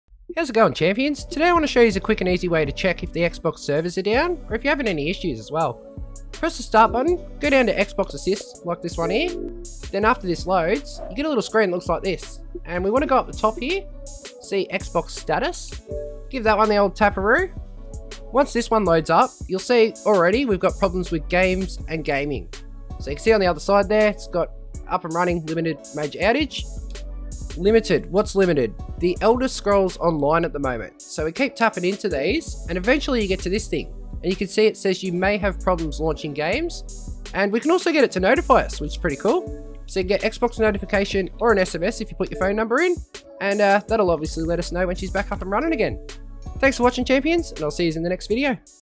Xbox Servers Down? Check here..
Here's a quick and simple way to check if the Xbox Servers are down. Hope it helps someone!